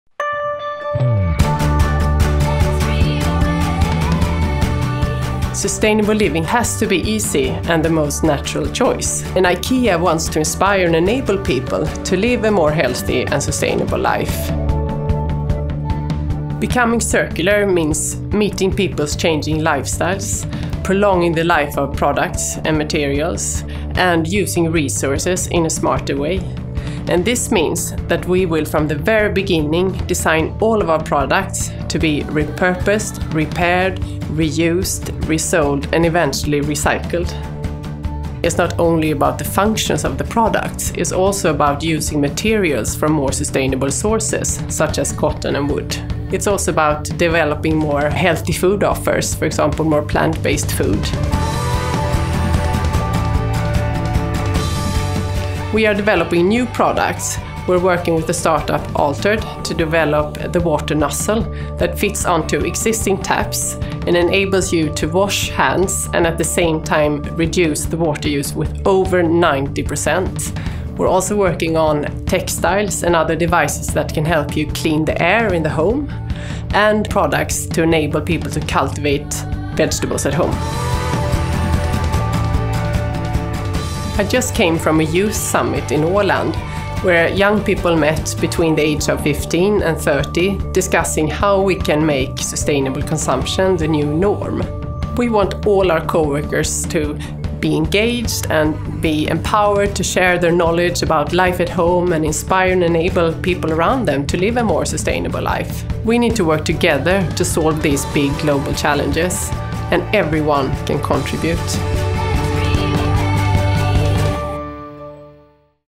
IKEA facts and figures 2018: climate positive
The fight against climate change needs a whole new way of thinking... and a whole new way of doing business. See how we’re doing our bit to help, and what you can do too.